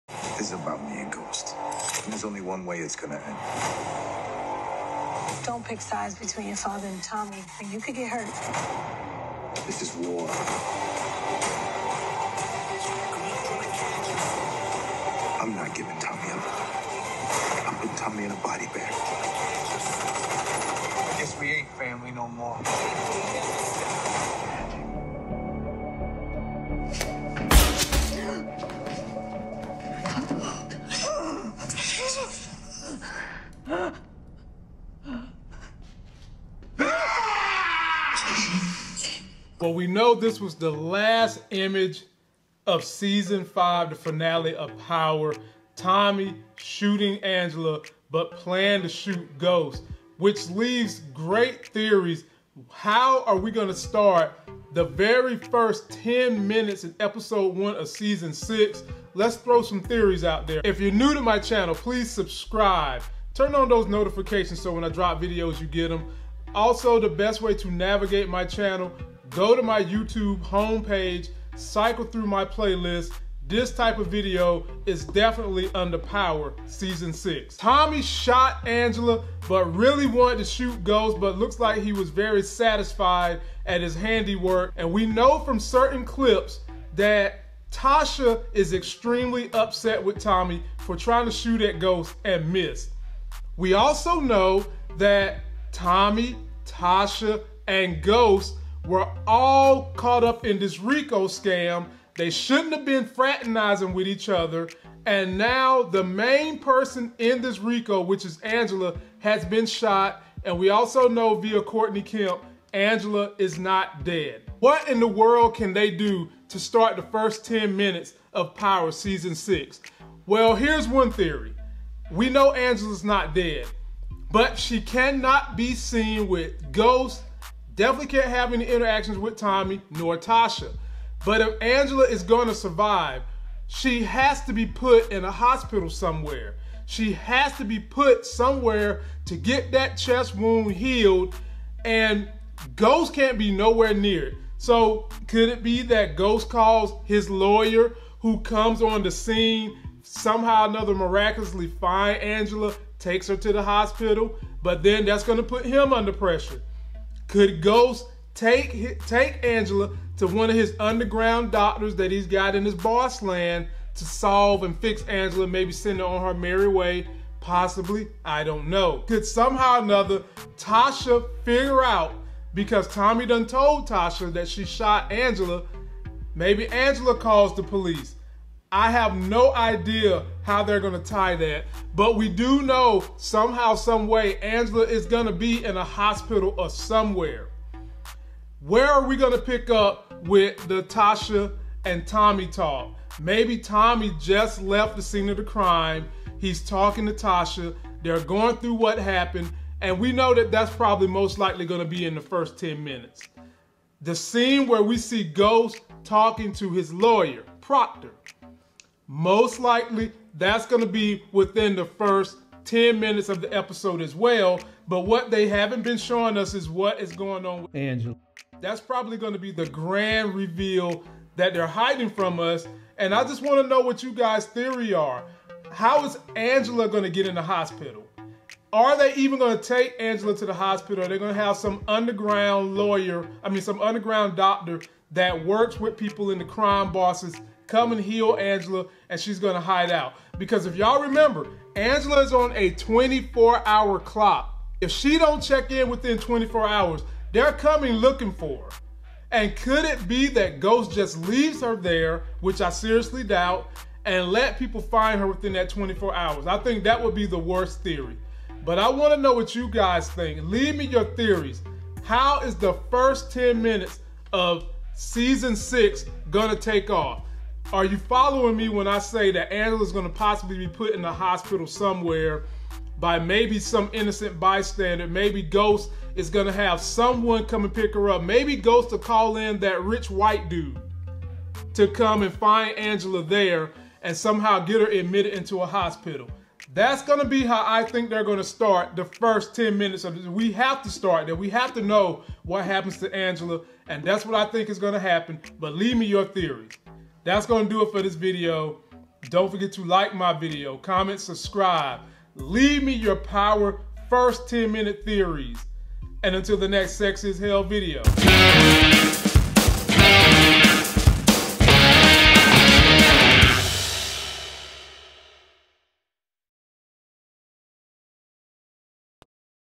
Power Season 6 Predictions | Power Season 6 Episode 1 Predictions | Angela Lands In The Hospital
Power Season 6 Predictions | Power Season 6 Episode 1 Predictions | Angela Lands In The Hospital and What Else Happens in The First 20 Minutes.

In this video, we cover the theory that Angela is alive and will find herself in a hospital to start the first 20 minutes of episode 1 in season 6 of Power.

Hire Me